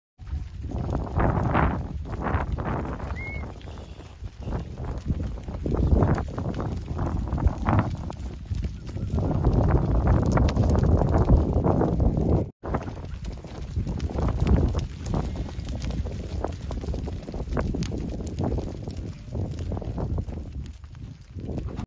ضان حريات حرش فاخرة عددها 19 راس السوم : 870 )) 0566974950
غنم حريات حرش فاخرة عددها 19 راس والغنم دفيع ومضاريع وفيها شيء قرايب واصلات على ولاد والغنم شبب في اسنانها وغنم مراح واحد والغنم فرصة للي يدور على غنم مقنوي 🔥🏅
السوم : 870 والبيع ماهو بعيد للزبون الصامل .
الموقع : مكة عمق .